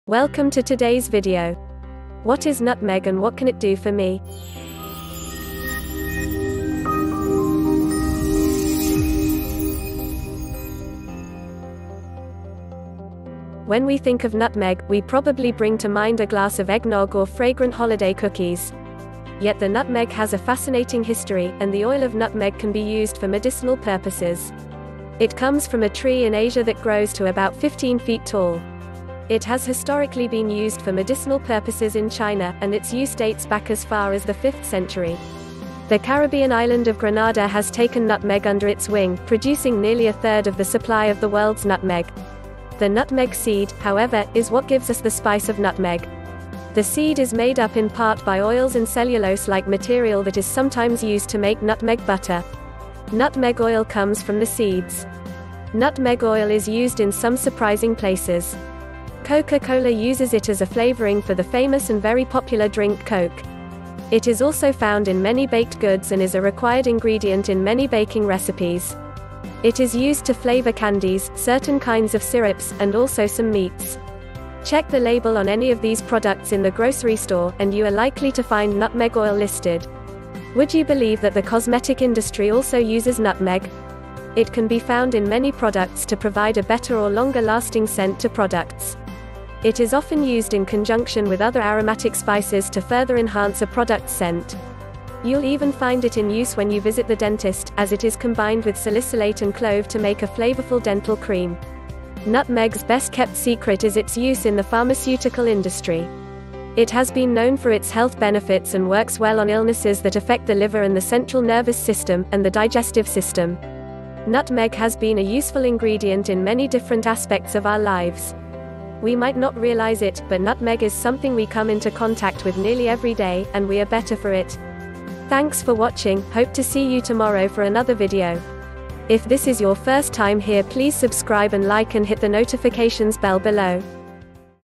What Is Nutmeg And What Can It Do For Me?
Below Brief Summary of Video
When we think of nutmeg, we probably bring to mind a glass of egg nog or fragrant holiday cookies.  Yet the nutmeg has a fascinating history, and the oil of nutmeg can be used for medicinal purposes. It comes from a tree in Asia that grows to about fifteen feet tall.

It has historically been used for medicinal purposes in China, and its use dates back as far as the fifth century.  The Caribbean Island of Grenada has taken nutmeg under its wing, producing nearly a third of the supply of the world’s nutmeg.

The nutmeg seed, however, is what gives us the spice of nutmeg.  The seed is made up in part by oils and cellulose like material that is sometimes used to make nutmeg butter.